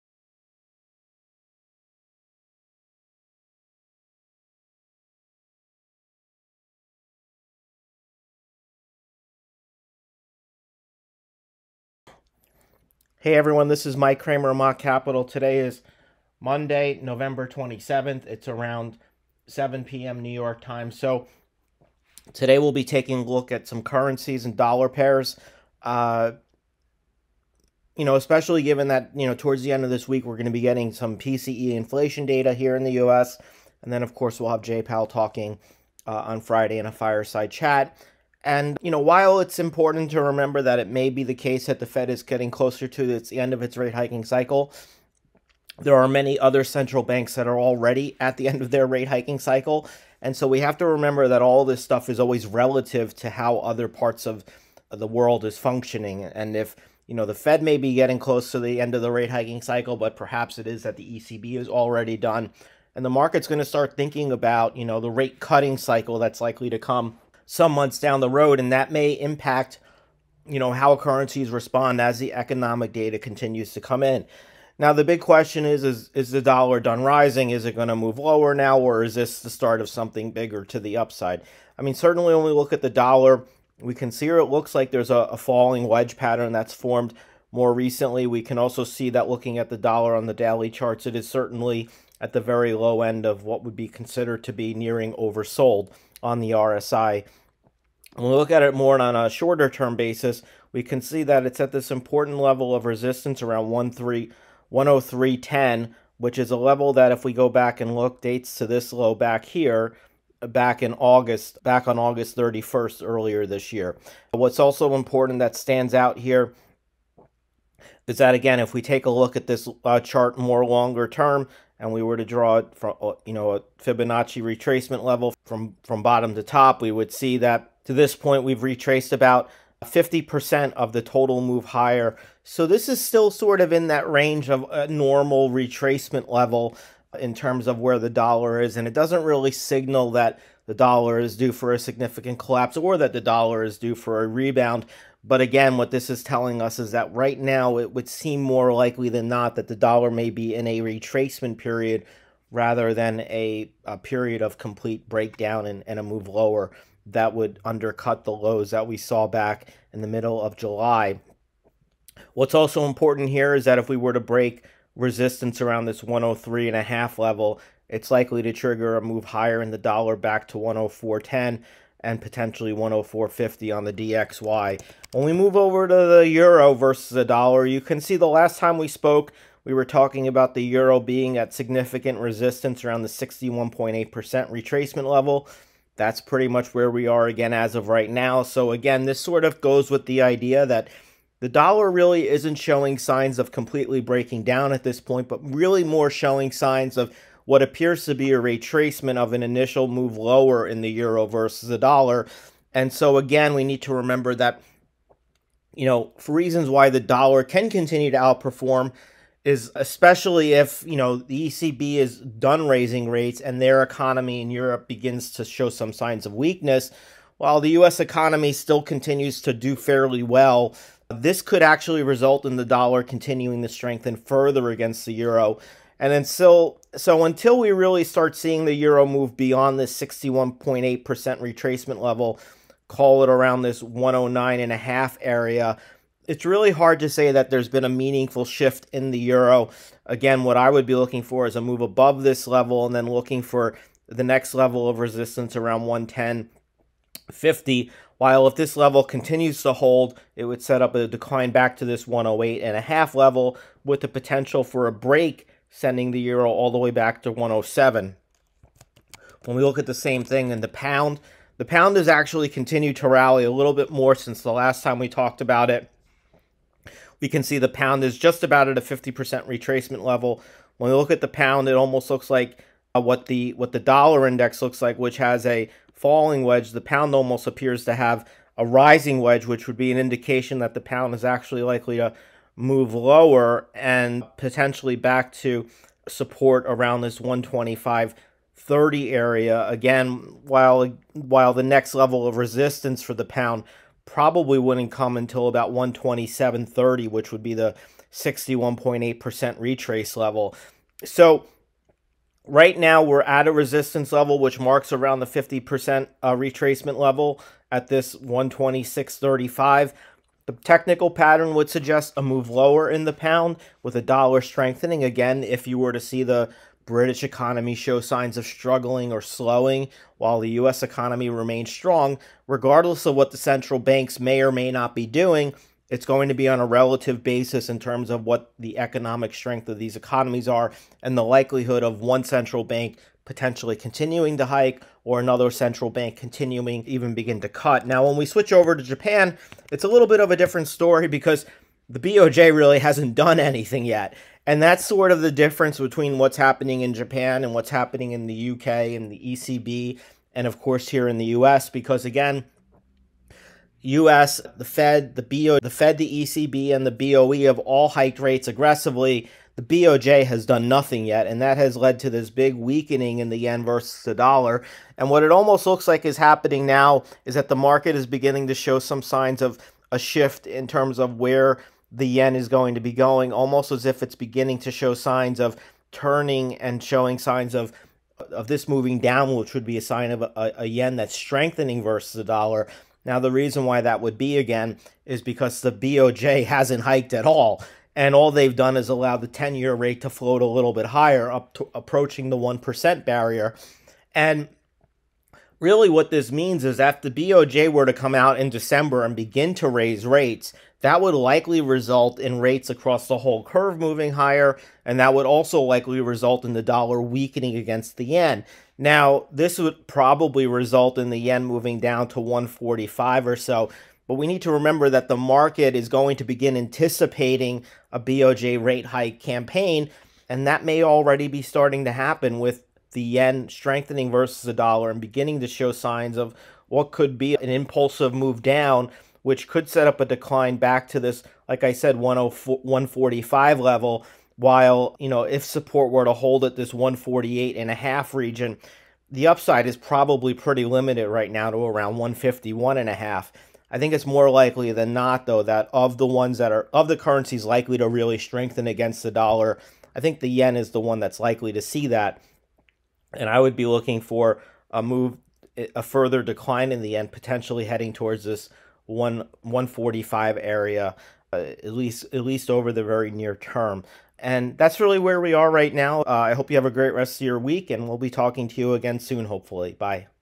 Could the US dollar strengthen against the pound and yen? | Michael Kramer
Michael Kramer, founder of Mott Capital Management, shares his short-term forecasts for major dollar pairs, outlining their possible directions of travel.